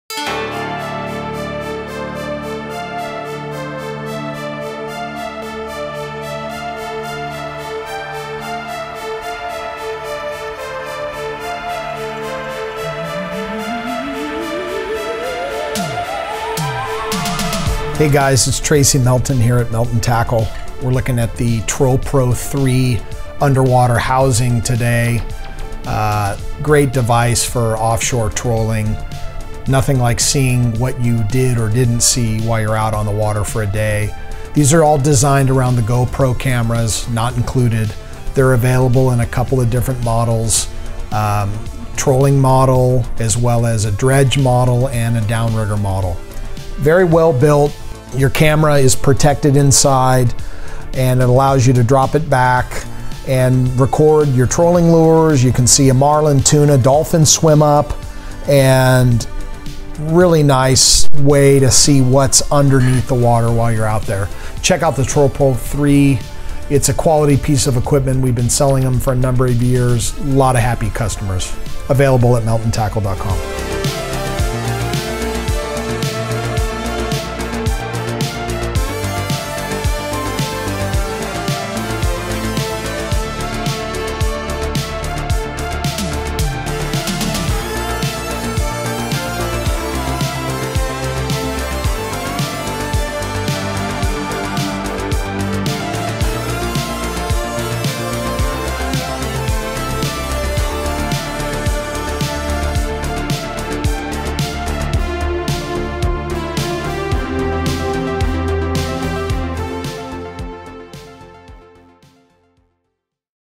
TrollPro3, the ultimate Underwater video recording
Expand the versatility of your GoPro camera to produce stunning, professional quality, underwater video recording of your trolling spread and fishing action.

This latest model is designed to support the line of GoPro HD Hero cameras, rapidly gaining popularity with boaters and sports fishermen as one of the most affordable, high performance, high definition point-of-view video cameras with a waterproof housing. The hydrodynamic design and keel weighting of the TrollPro3 housing provides a highly stable platform at trolling speeds for clear, wide field of view video recording on the cameras removable data card, to produce high definition, professional-quality underwater video recordings of your lures, baits and fishing action behind and below the boat, adding a whole new dimension to your fishing excursions.